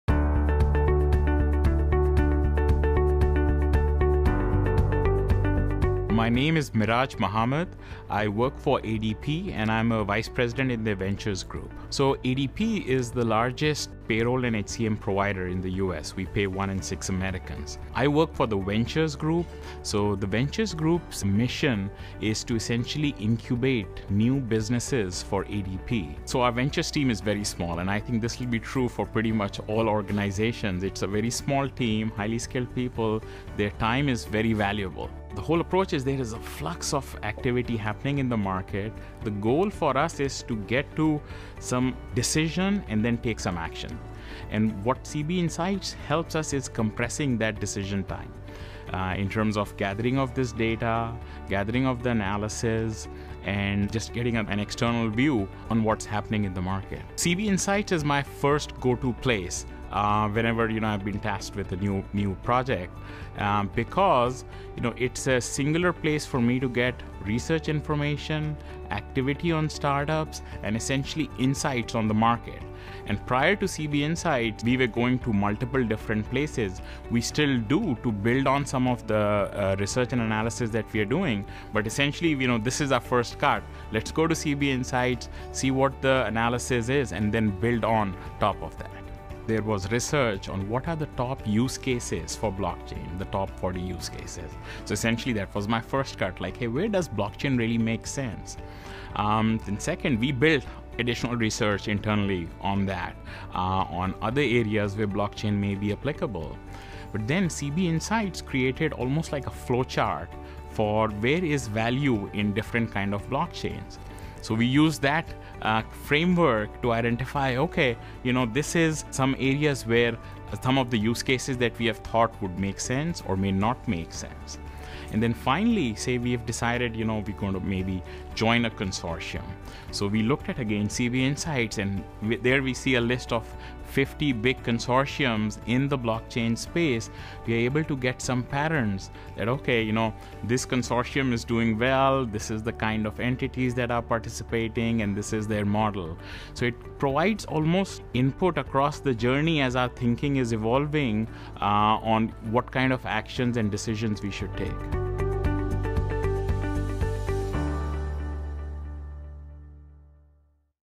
Customer Story: How ADP makes faster, data-driven decisions with CB Insights
ADP is the largest Human Capital Management provider in the US, managing payroll for one in six Americans. See why they trust CB Insights to shape strategic decisions and keep ahead of disruption